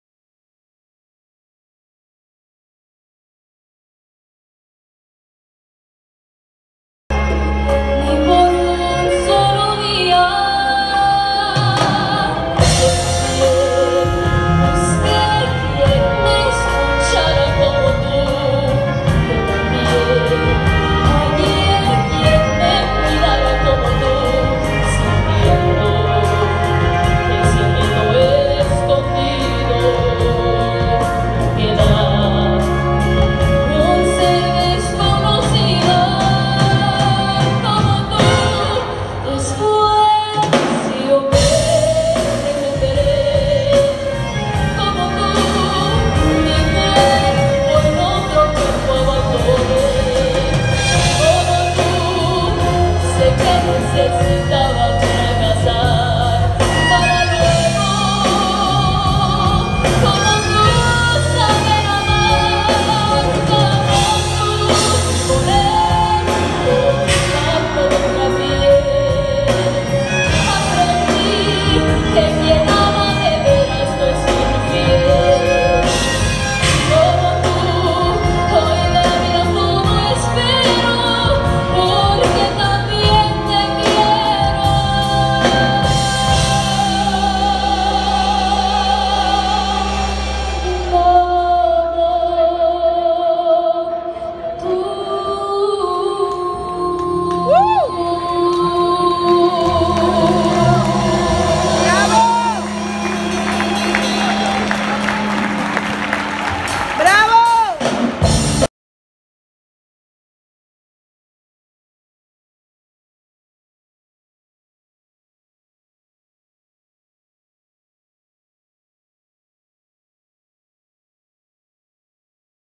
Tiffany Galaviz -FESTIVAL OTI USA 2017
Una excelente participación de Tiffany Galaviz dentro de la apertura del Festival OTI USA  2017
The Palace Theater , Los Angeles, CA